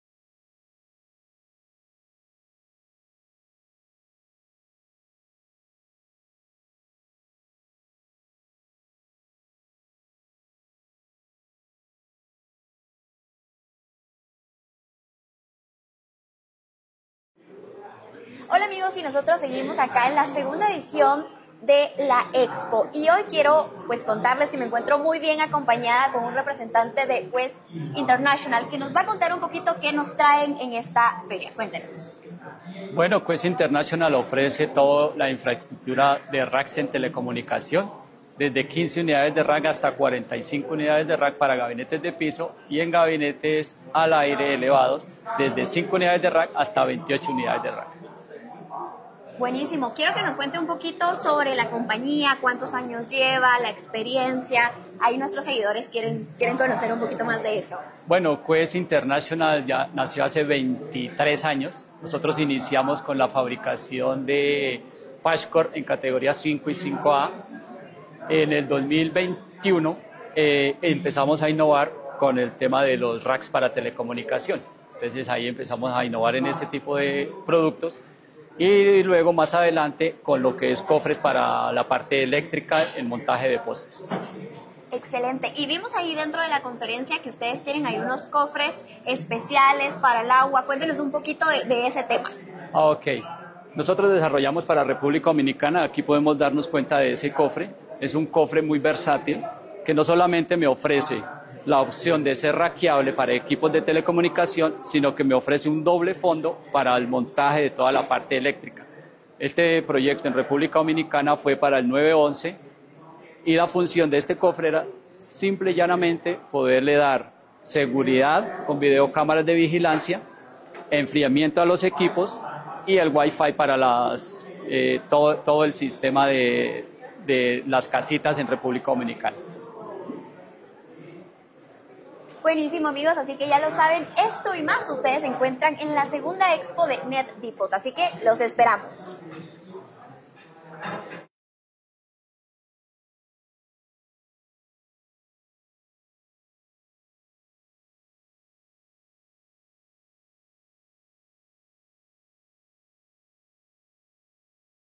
SEGUNDA FERIA EXPO NET DEPOT
Te presentamos los productos de Quest Internacional S.A con certicaciones UL, Retie e ISO90001 v15 para Telecomunicaciones y Eléctricos. Somos una empresa Colombiana y exportamos a diferentes paises nuestro productos de la marca QUEST INTERNATIONAL.

Visita nuestro punto de venta
Cra 6 # 19-40 Local 03 B/San Nicolás Cali-Valle
Para mayor información: